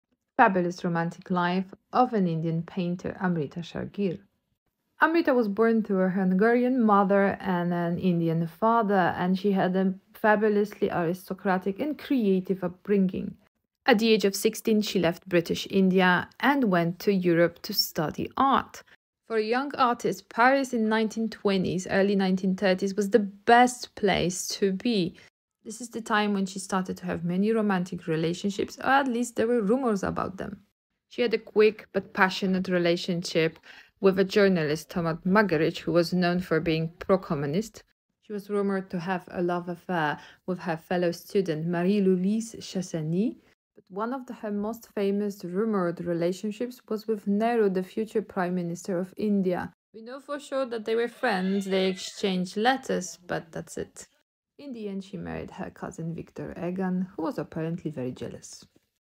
Romantic life of an Indian modernist painter | Amrita Sher-Gil in 1920s 1930s
Amrita Sher-Gil the modernist Indian painter of the British India